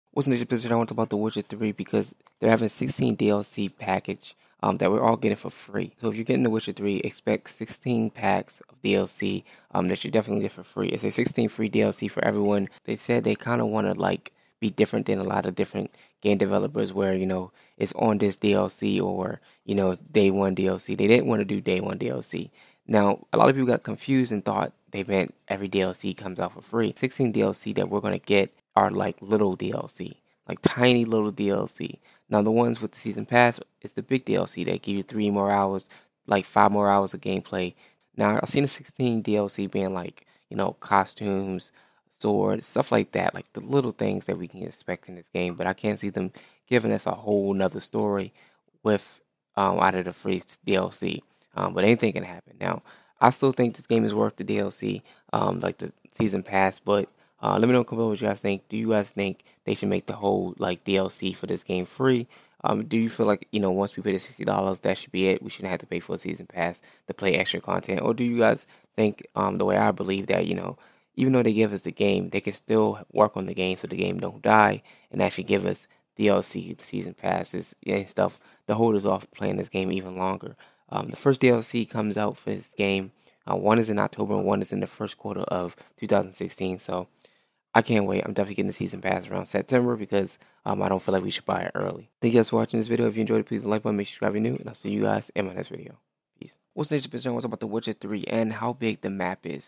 The Witcher 3: Wild Hunt 16 FREE DLC
Im sure we all are used to Day 1 DLC or hate buying DLC when games used to have everything on disc when it came out. But with the Witcher we are getting around 16 FREE DLC. Tell me what you guys think about this.

PSN: jelanidozier